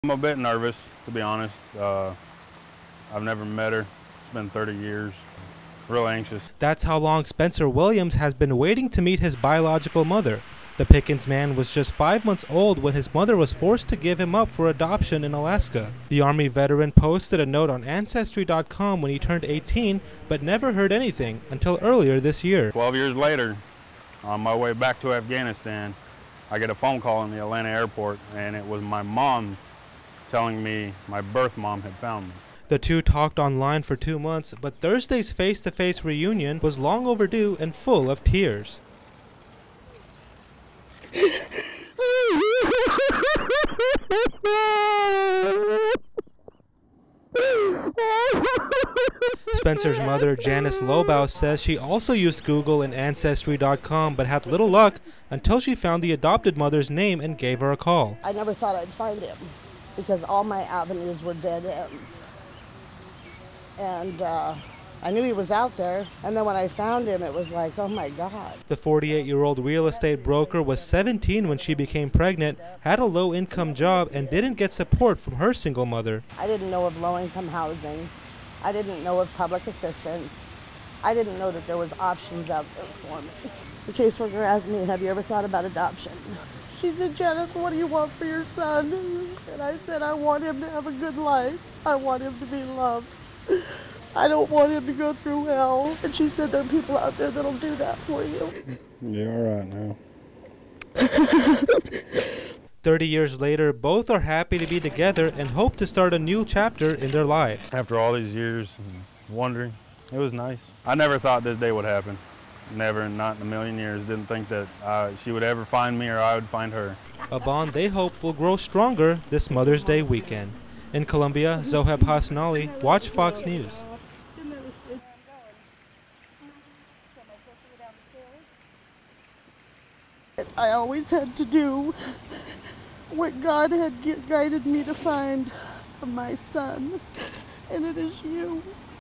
Adopted son meets biological mother after 30 years
Mother's Day came a few days early for one South Carolina man.